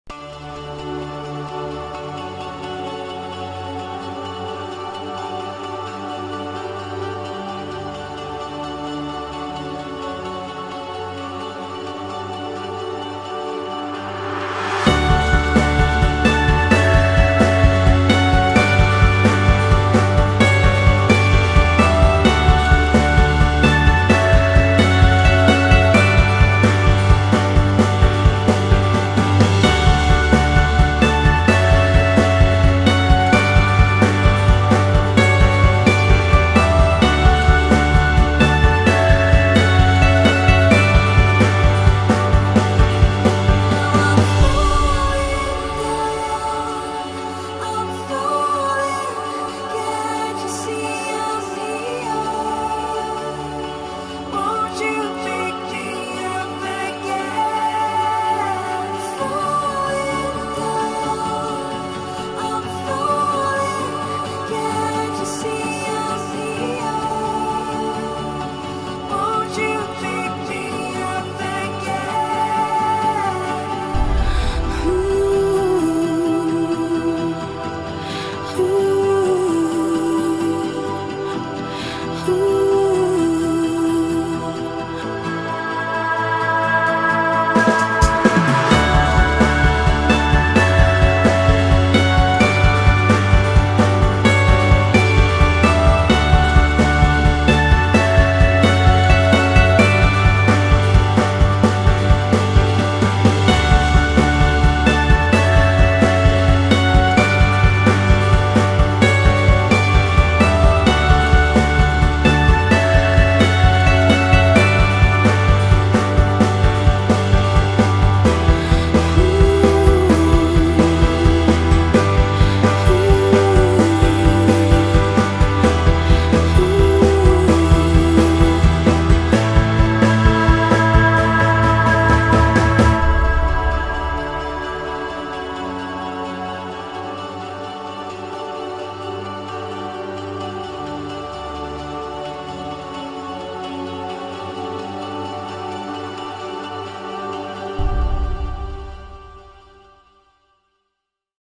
Our Backyard in 4K video
test flight of my first drone, Mavic Air 2